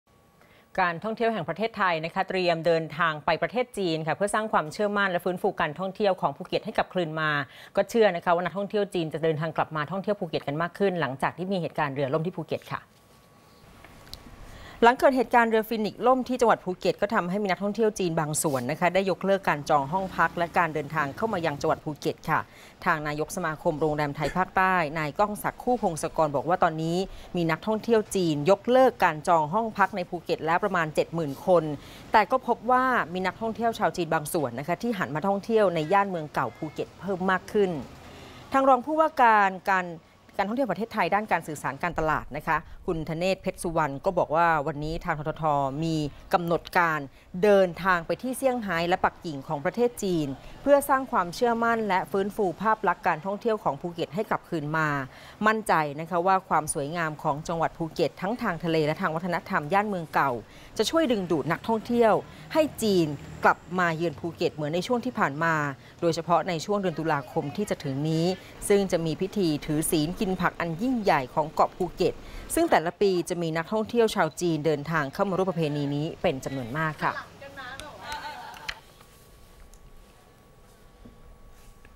ททท.เตรียมเดินทางไปจีน สร้างความเชื่อมั่นการท่องเที่ยวภูเก็ต
ททท.เตรียมเดินทางไปจีนเพื่อสร้างความเชื่อมั่นและฟื้นฟูการท่องเที่ยวของภูเก็ตให้กลับคืนมา  เชื่อนักท่องเที่ยวจีนจะเดินทางกลับมาท่องเที่ยวภูเก็ตมากขึ้น